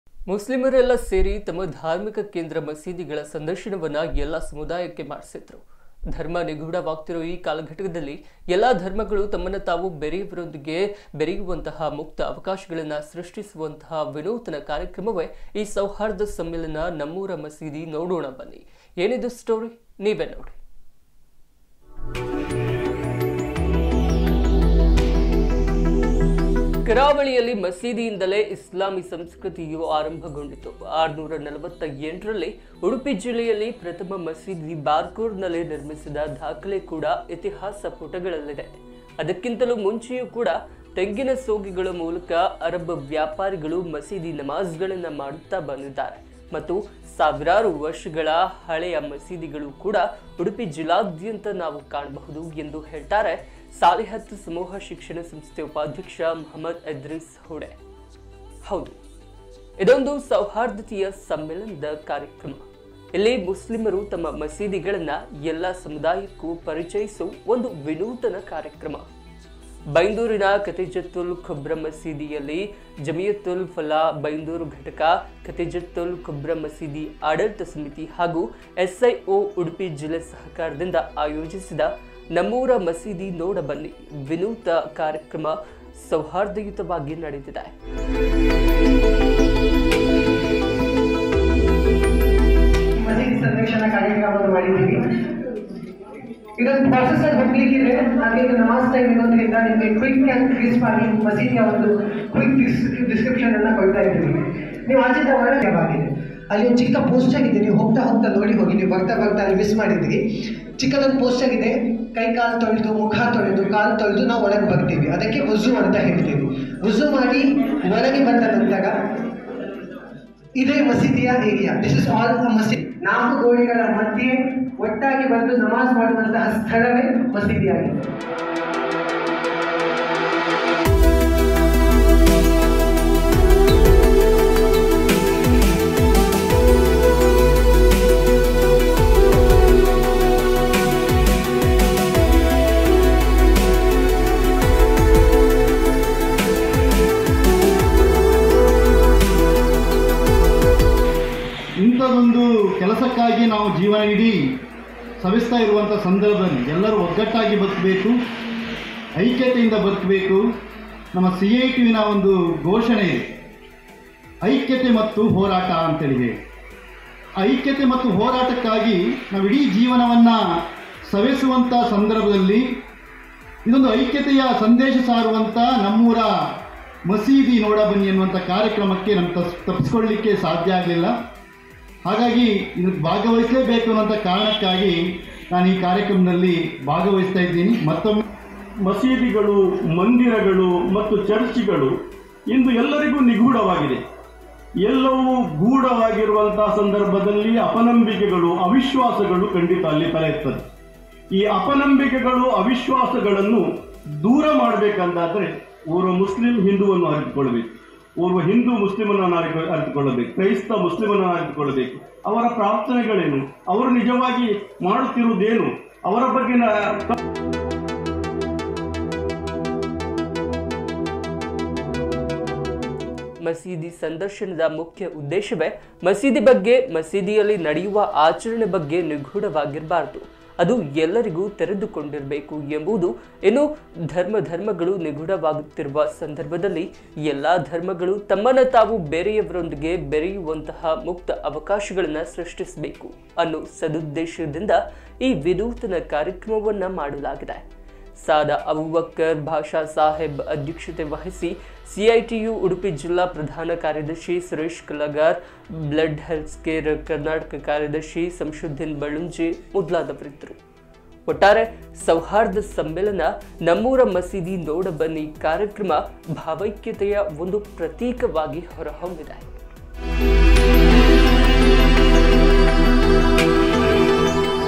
ಈ ಸೌಹಾರ್ಧ ಸಮ್ಮಿಲನ ನಮ್ಮೂರ ಮಸೀದಿ ನೋಡೋಣ ಬನ್ನಿ
ಧರ್ಮ ನಿಗೂಢವಾಗ್ತಿರೋ ಈ ಕಾಲಘಟ್ಟದಲ್ಲಿ ಎಲ್ಲಾ ಧರ್ಮಗಳು ತಮ್ಮನ್ನು ತಾವು ಬೇರೆಯವರೊಂದಿಗೆ ಬೆರೆಯುಂತಹ ಮುಕ್ತ ಅವಕಾಶ